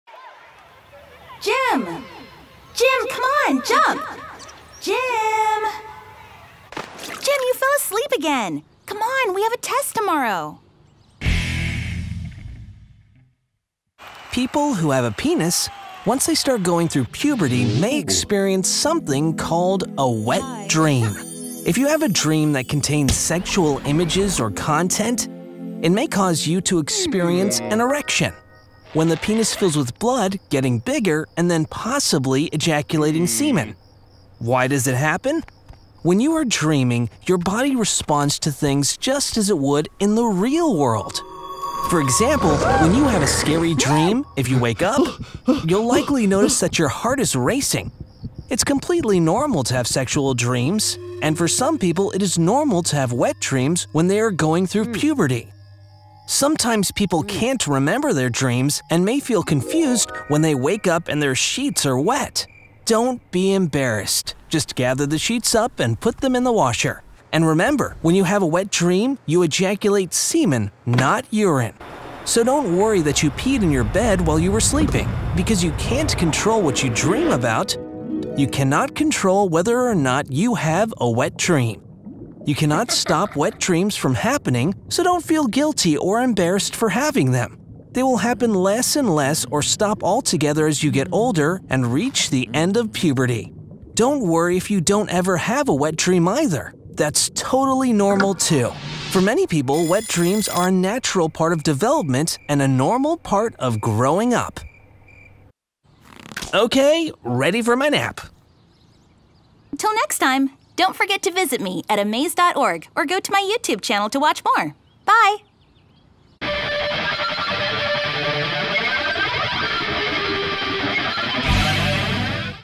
What is a Wet Dream? (Nocturnal Emission)
There are all sorts of dreams and when you're hitting puberty, you might experience a wet dream.  They may happen often or not at all, and that's all totally normal. Watch to learn more!
Follow us on Snap and Insta for more info about puberty and growing up.

[AMZ-059]